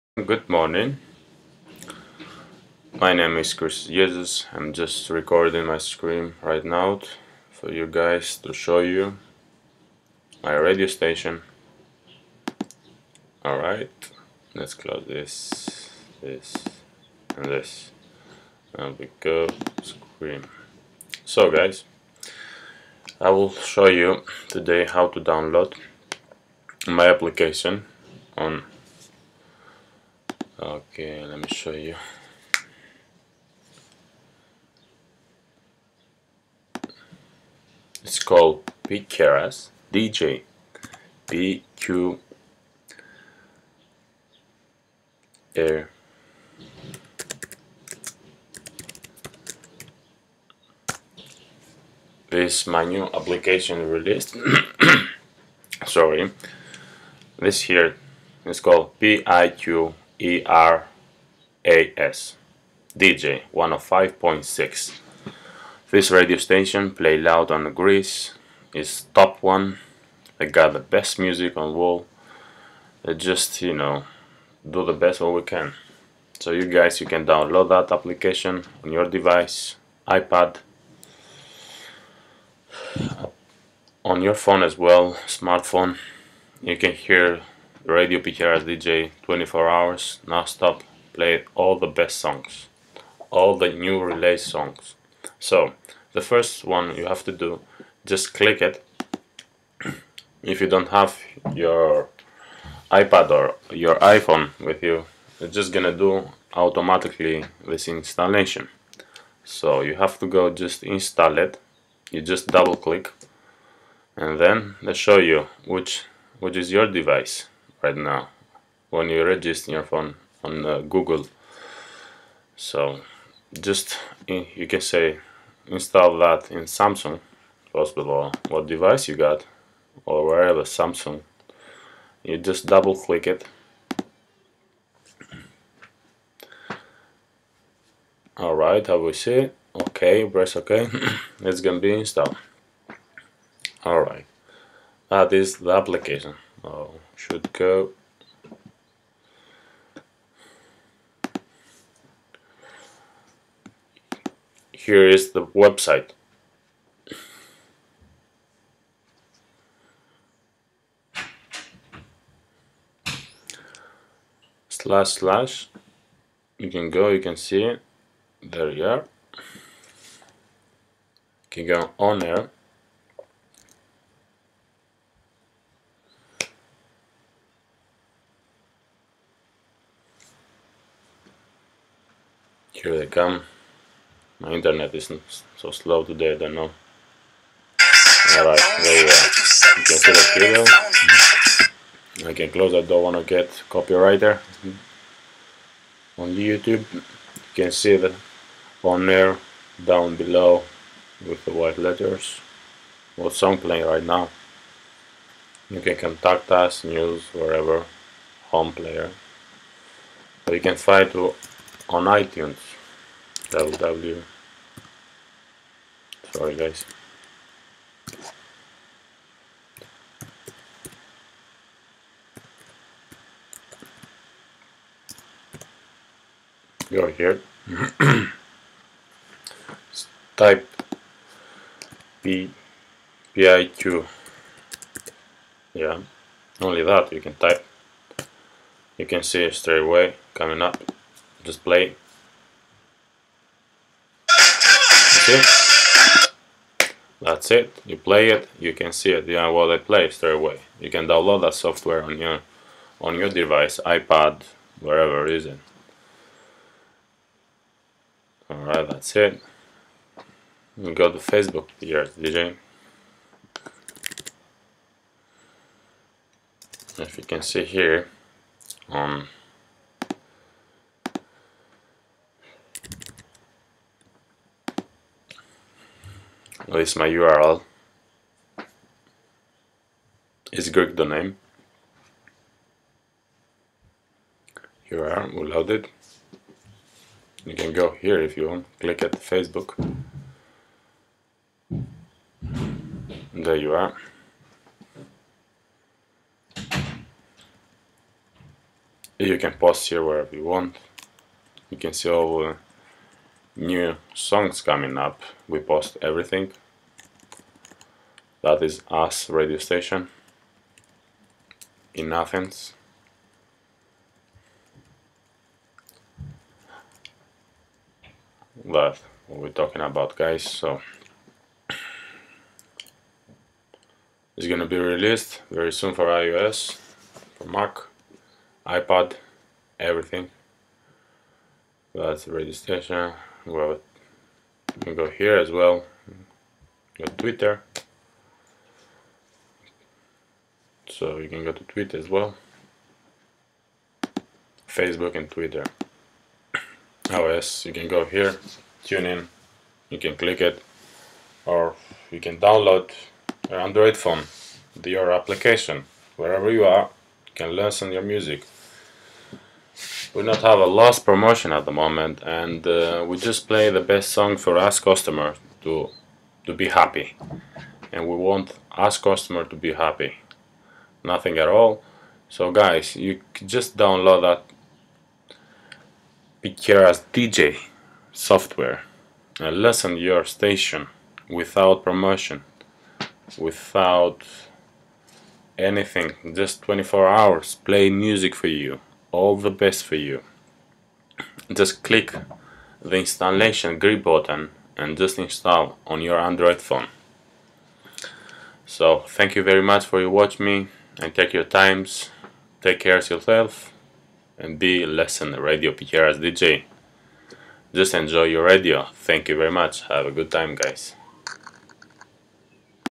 How to listen online Radio Deejay 105,6
Welcome to RADIO  Piqeras Deejay 105,6.

Now you can enjoy more music HOT Today in Android smartphone and iOS app (for iPhone, iPad and iPod)./ or tablet.

On the first page of the application "on air" you can

 To stop and restart the program (Play / Pause),

Send e-mail to the studio when you hear P Deejay,

You call your top  producer at the time of the broadcast,
To access the Facebook page of Piqeras Deejay,

To follow the Piqeras Deejay on Twitter,
 Listen to the program in background mode,

To see the song title, artist name and photograph
album (where available) is Now On Air

 Application supports alarm messages (Notification messages), where you can see the song title, artist name is Now On Air, but to go
directly to the application.

 The application adapts depending on the orientation of your device (horizontal or vertical).

* The app features Real Multitasking background play and HD graphics.

station from the Browser application!

We wish
Good hearing.